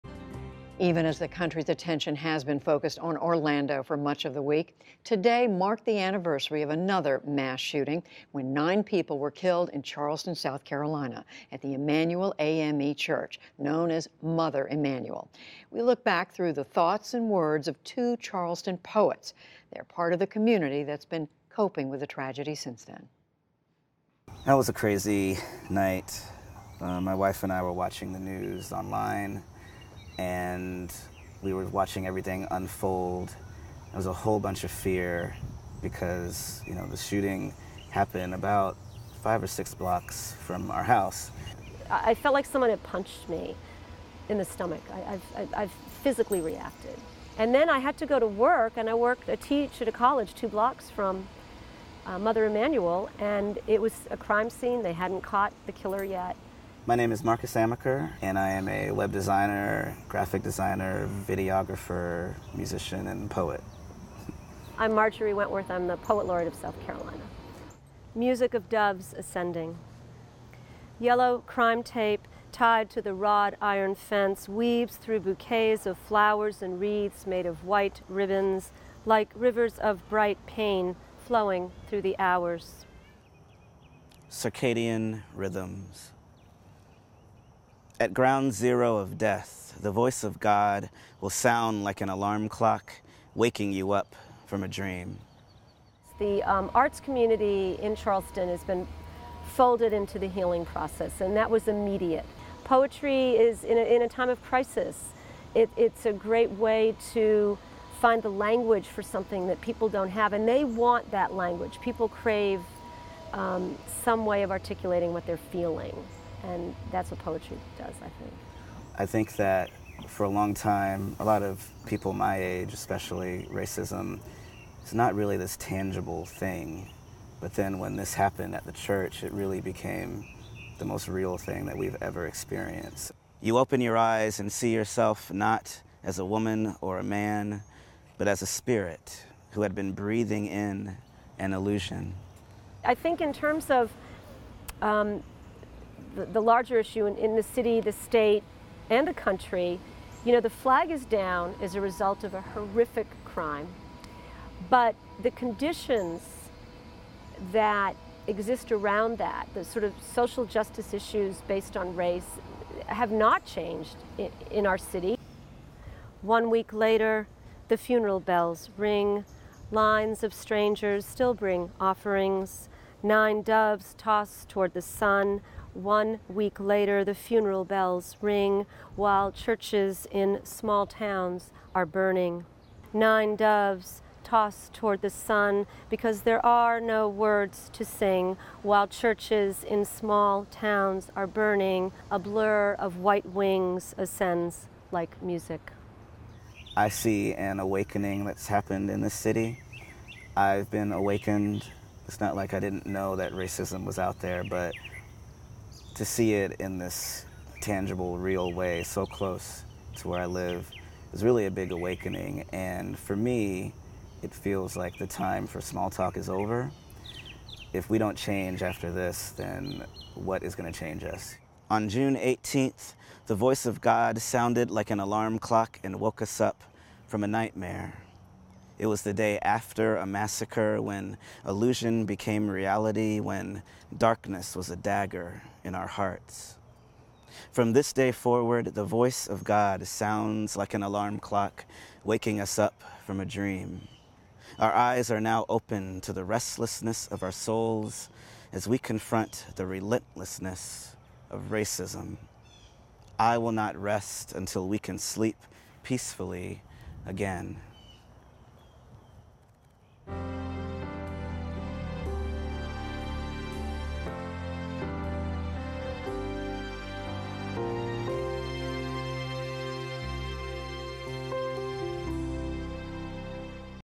On anniversary of church shooting, South Carolina poets offer healing through verse
Friday saw the first anniversary of the shooting at Charleston’s Emanuel AME Church, where alleged white supremacist Dylann Roof killed nine black churchgoers. Among those who struggled to come to terms with the tragedy were two local poets, musician and web designer Marcus Amaker and South Carolina Poet Laureate Marjory Wentworth, who share some of their reflections in verse.